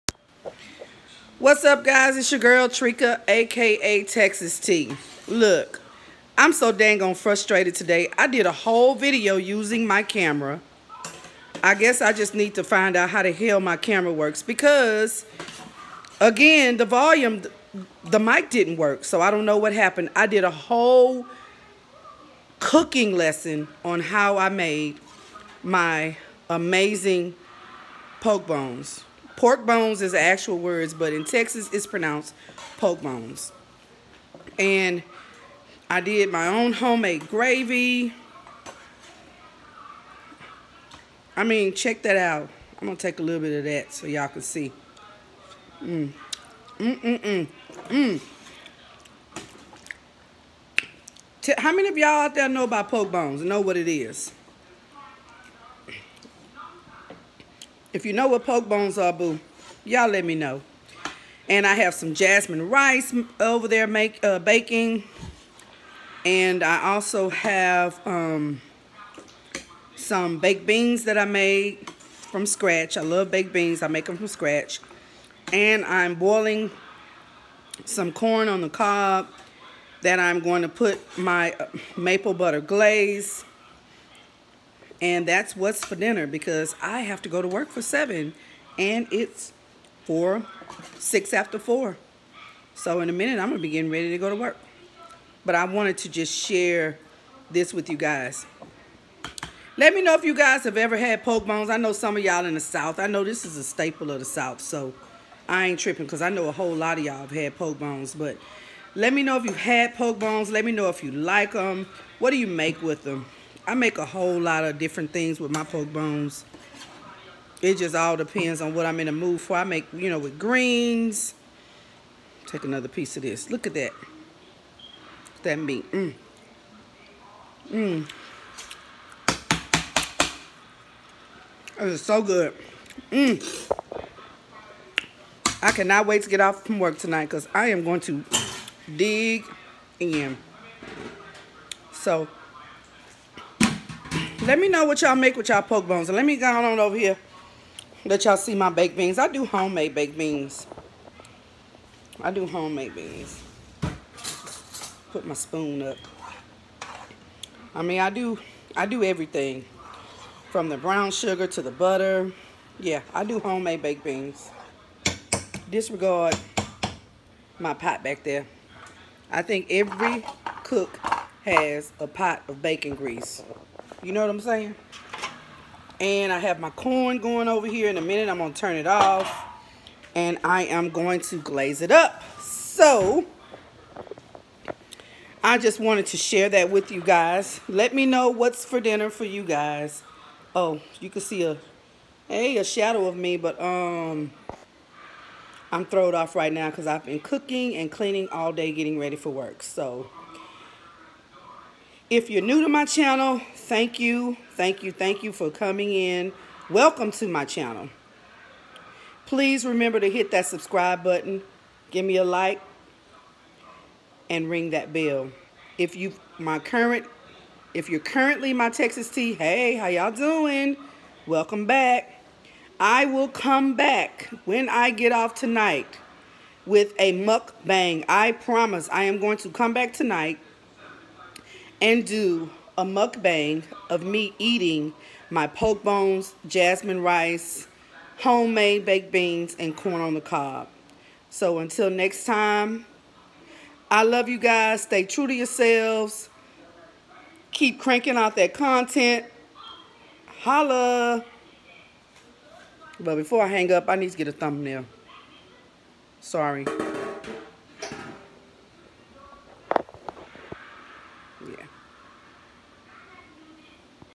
Smothered Pork bone homemade baked beans with maple glazed corn on the cob!
Hey my Texas Tees! I’m back with another teaser! Today I made smothered POK-BONES with homemade baked beans and maple glazed corn on the cob! This video shows my finished pork bones and baked beans. I’ll come back later with a mukbang! See ya later!